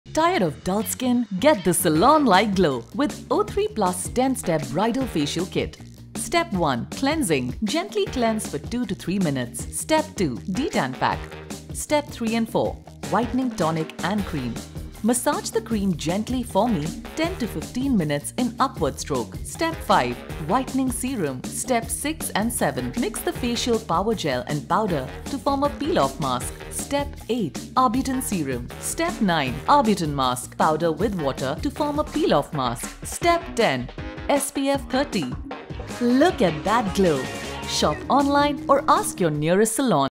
Skincare Routine for Brides - 10 Steps O3+ Bridal Facial Kit
Looking to achieve the perfect bridal skin that glows from within? The O3+ bridal facial kit is all you need to achieve flawless skin for your big day or any other special event. Providing a deep serum penetration, a peel-off mask, moisturization, and a healthy glow, this regime aims at giving you the healthiest glowing skin.

This kit is not only for brides, it is an ultimate radiance facial for all types of skin. It can be used by 18+

* Formulated in Italy by dermatologists.
* Suitable for dry uneven skin.
* Enriched with purest vitamin c brightening.
* Evens out and gives a radiant skin

* O3+ Bridal Facial Kit for Radiant & Glowing Skin
* O3+ Bridal Facial Kit Oxygenating Glow Skin
* O3+ Bridal Facial Kit Vitamin C Glowing Skin